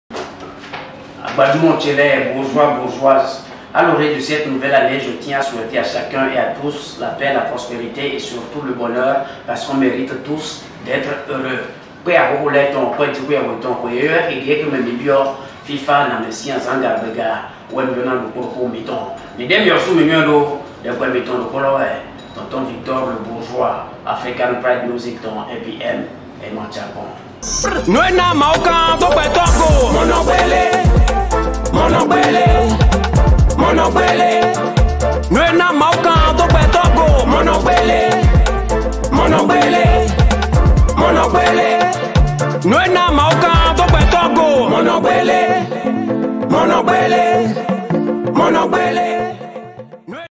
TONTON VICTOR - Meilleurs vœux 2022
Message de Tonton Victor à l'orée de la nouvelle année